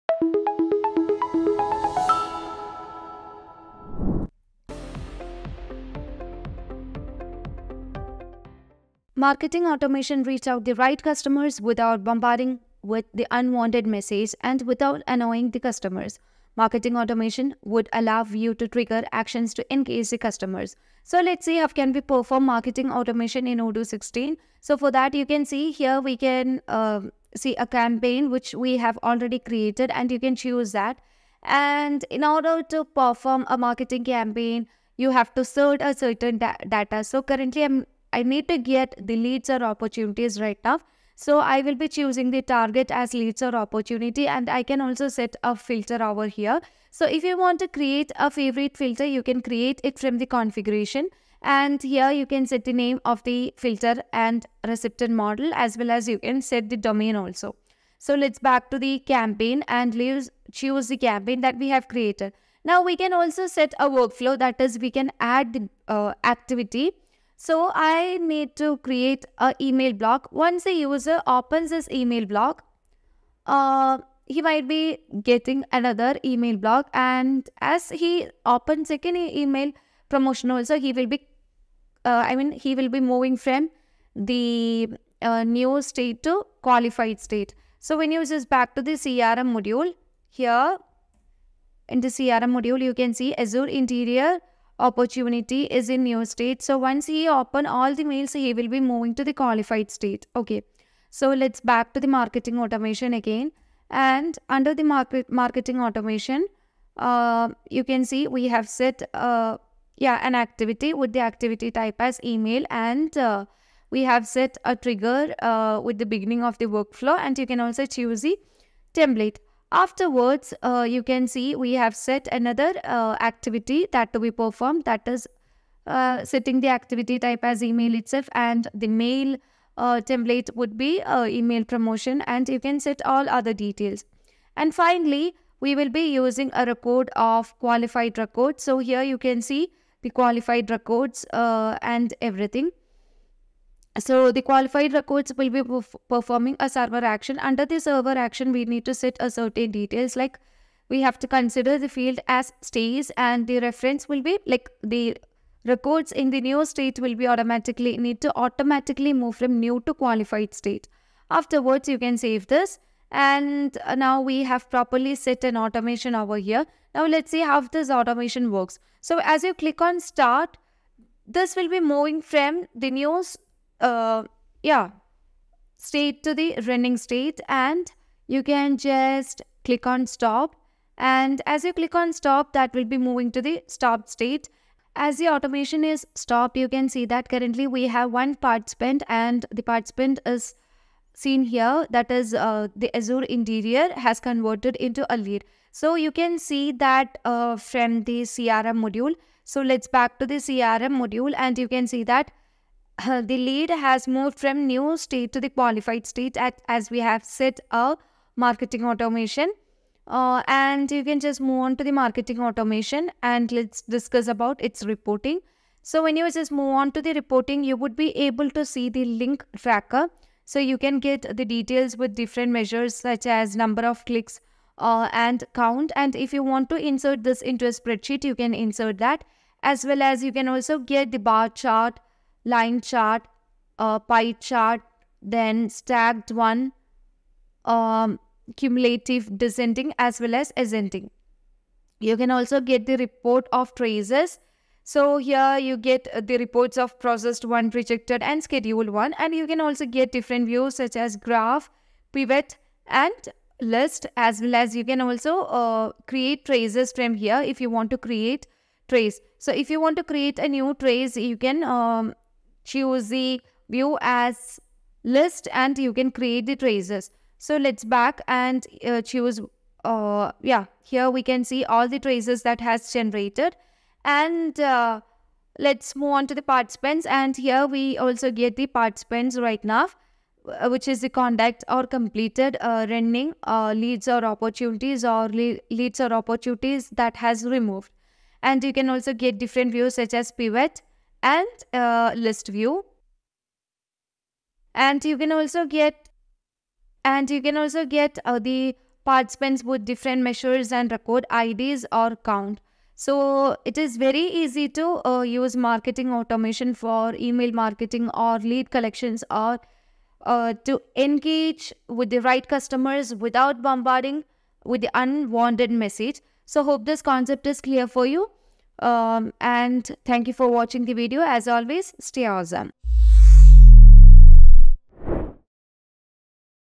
Marketing Automation in Odoo 16
Marketing automation refers to the use of technology and software to automate various marketing tasks and processes. This approach streamlines and optimizes marketing efforts, making them more efficient and effective.

Marketing automation can be applied to various aspects of marketing, including email marketing, social media marketing, lead generation, customer segmentation, and more.